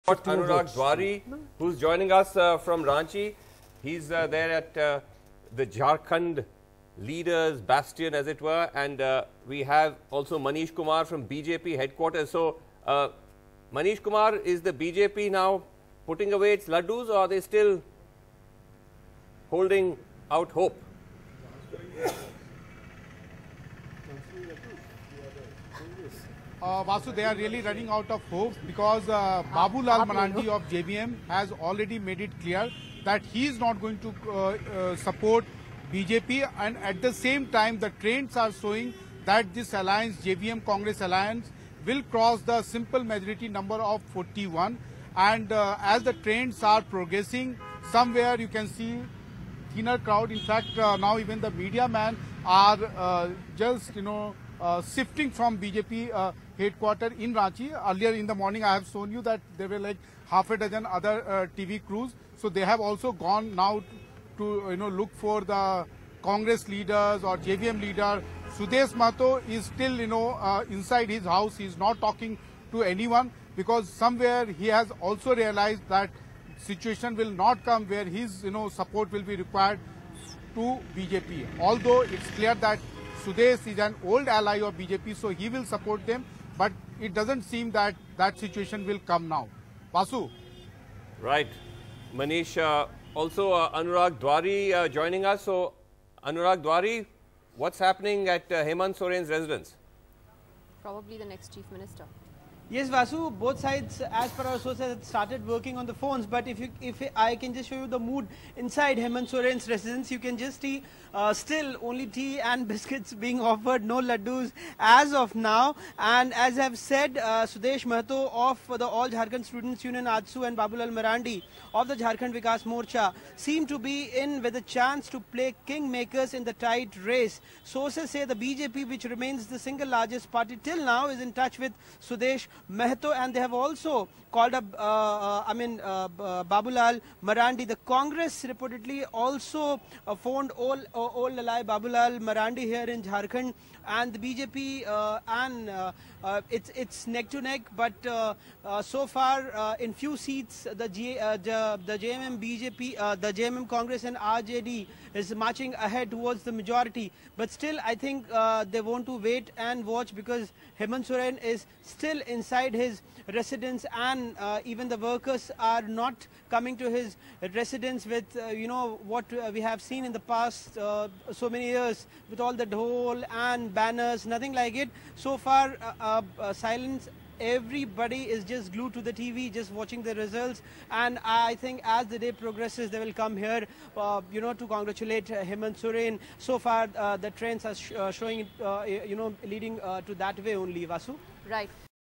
Jharkhand Election Results: JMM-Congress Alliance Ahead In Tight Race For Jharkhand
Jharkhand Election Results 2019: The Jharkhand Mukti Morcha (JMM) and Congress alliance took a lead over the ruling BJP as votes cast for the Jharkhand election were counted on Monday. At least three exit polls have predicted a win for the Congress-JMM-Rashtriya Janata Dal (RJD) alliance. Elections were conducted in 81 assembly seats of the state in five phases from November 30 to December 20. In the Lok Sabha elections held earlier this year, BJP had won 11 seats out of 14 in the state.